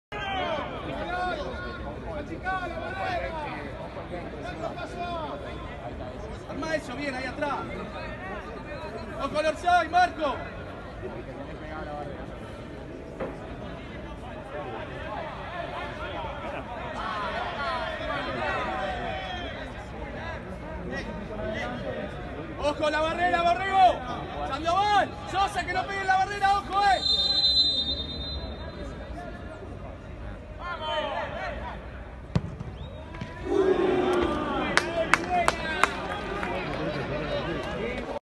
Juan Manuel Olivares y su Maravilla en cada pelota detenida
⚽️ Bajo un cielo gris en el barrio de Núñez, Defensores le ganó a Dálmine con goles de Sandoval y Borrego, ambos en el primer tiempo. Los dirigidos por Felipe De la Riva suman tres puntos importantes que los deja expectantes entre lo más alto de la tabla.

El partido se disputó el pasado viernes 10 de septiembre.

Vía RománTaccone para ÁreaAscenso

Gentileza